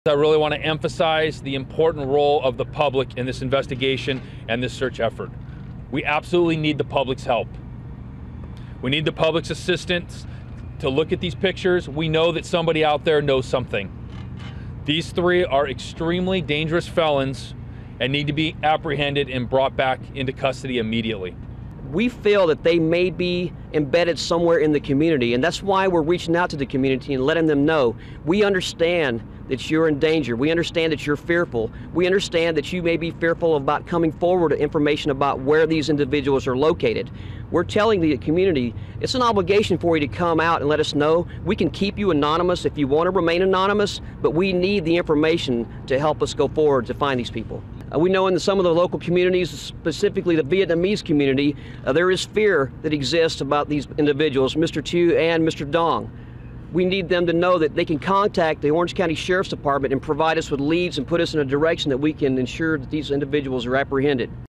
Appeal to Vietnamese Community to Find Inmates
Authorities believe at least two of the three men who escaped from an Orange County jail are still in the region. The area has a large number of Vietnamese-Americans and officials believe two of the men may be "embedded" in the community. (Jan. 25)